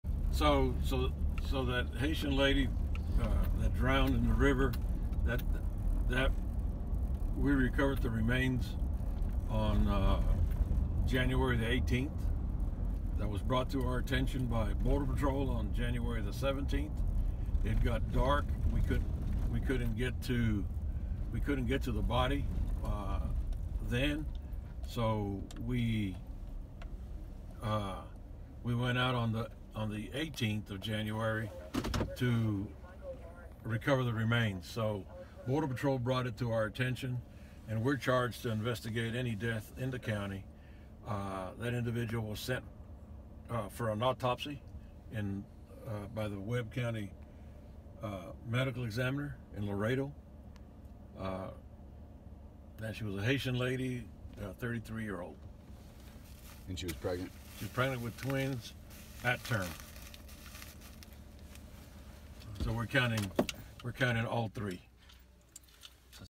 South Texas Sheriff: Migrants Are SURGING Into Area
In South Texas, Val Verde County Sheriff Joe Frank Martinez tells The Federalist that migrants are surging into the area, and a growing number are drowning while trying to cross the Rio Grande. Nine have drown since Jan. 18, compared to four all of last year.

The Federalist is a conservatarian web magazine focused on culture, politics, and religion. Be lovers of freedom and anxious for the fray.